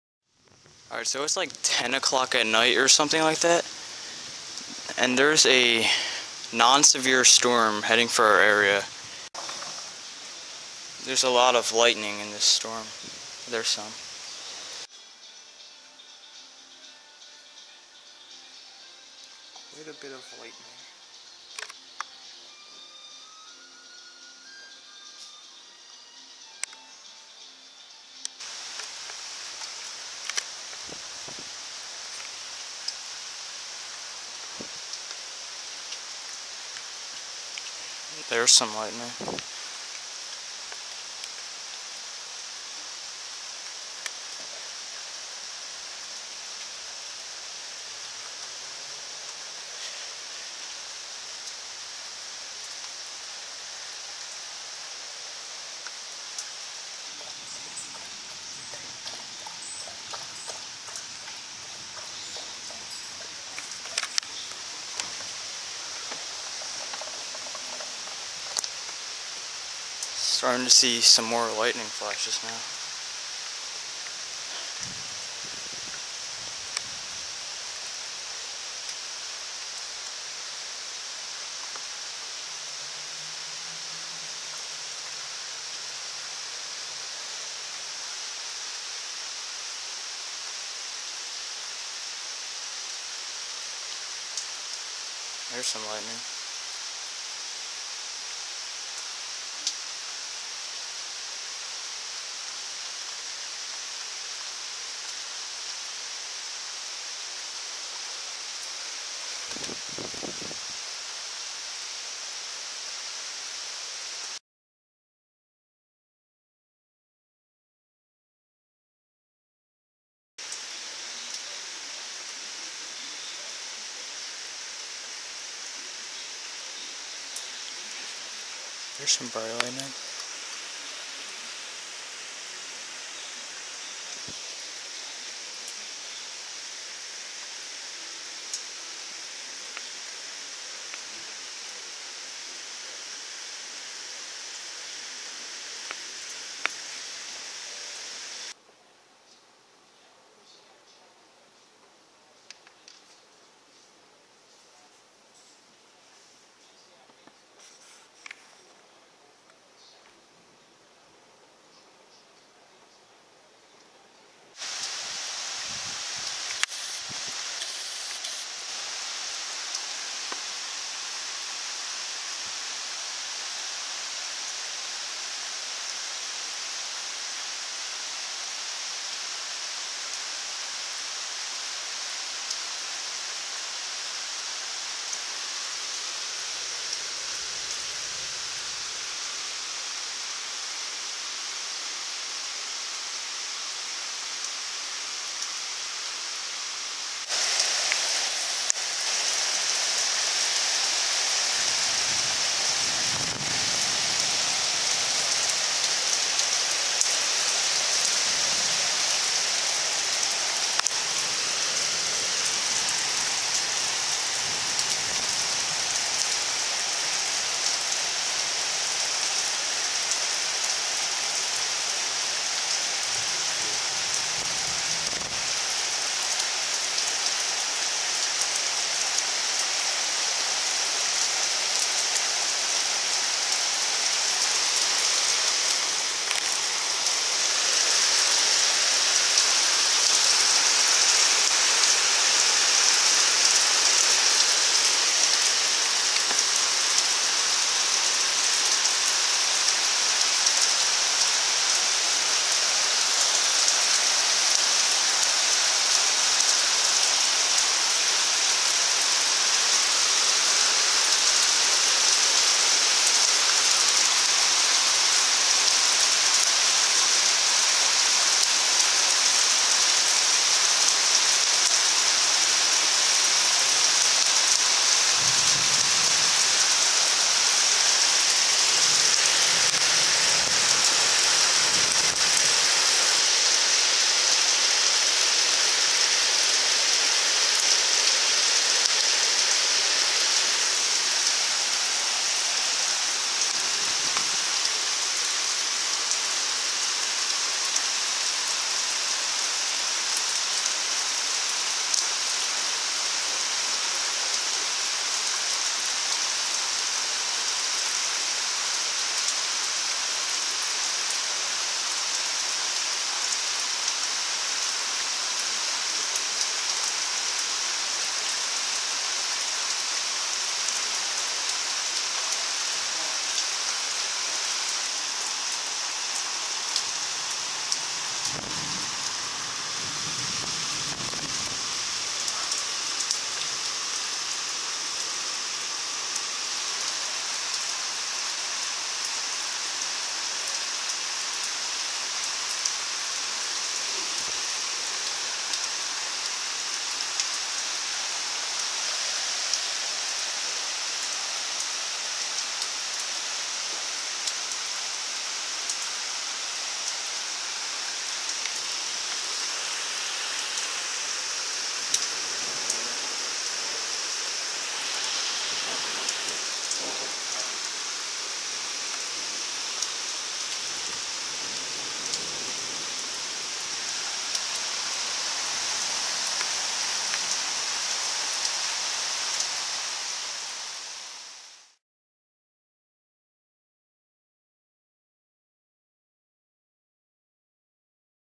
Passing By Storm At Nighttime! | 2018 Storms (5/13/18)
A passing by thunderstorm bringing some heavy rain on May 13th. Nothing too exciting, but just wanted to get this footage posted from a few weeks ago.